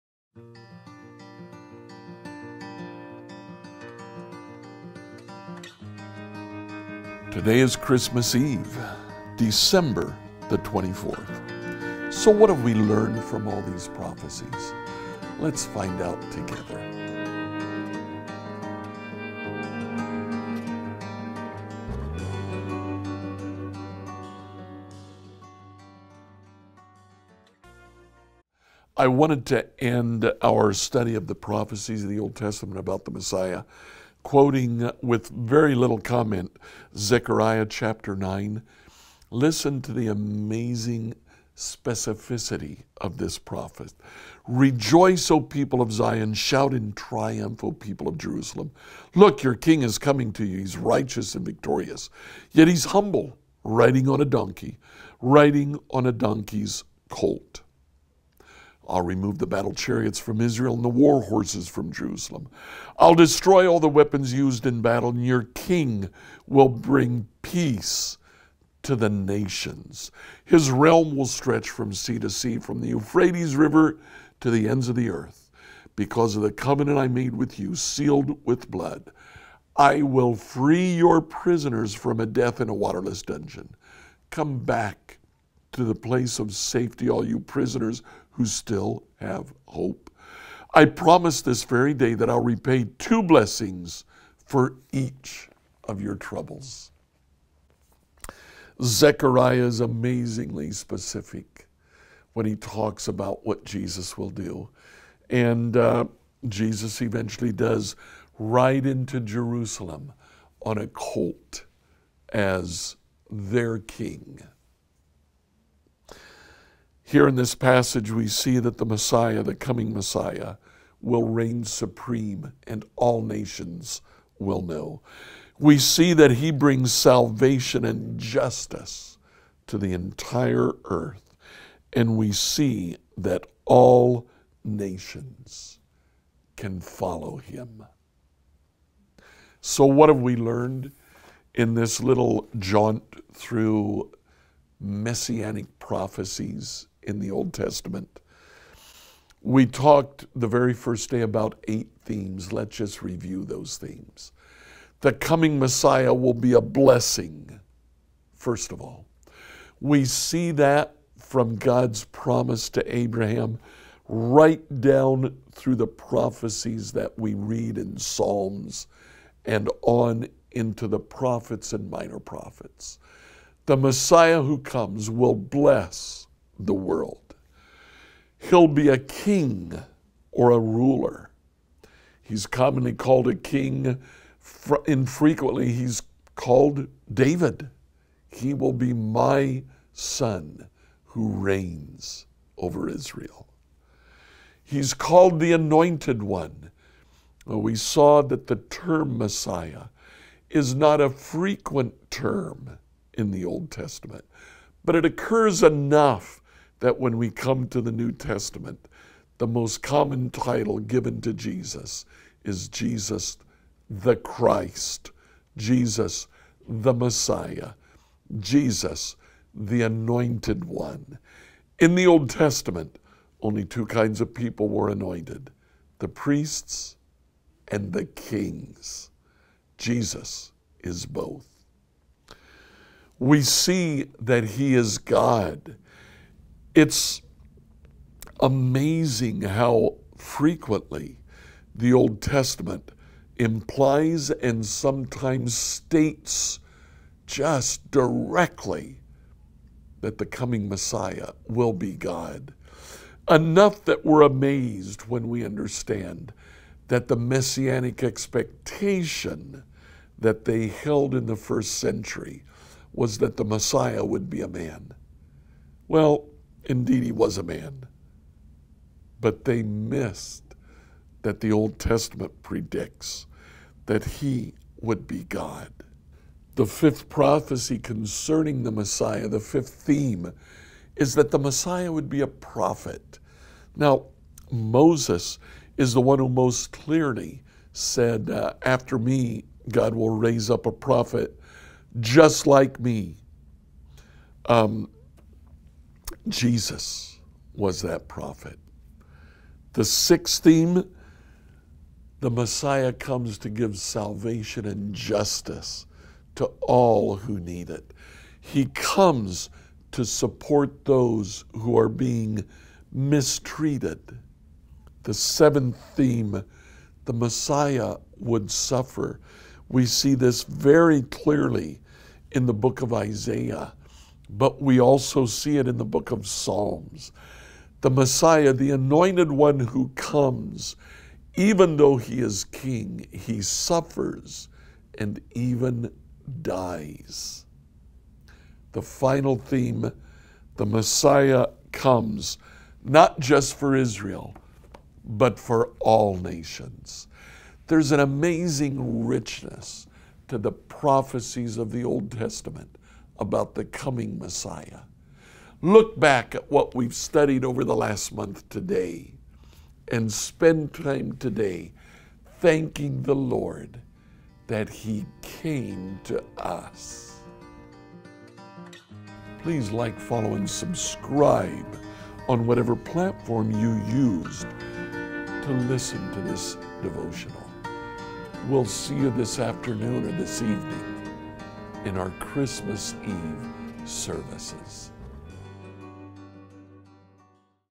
What have we learned? - December 24, 2021
December 24, 2021 - What have we learned?

In our little stroll through the Old Testament prophecies about Jesus, just what have we learned? Let’s find out together. When you finish, think back on what you’ve learned. What is the most important thing you take away from the Old Testament prophecies?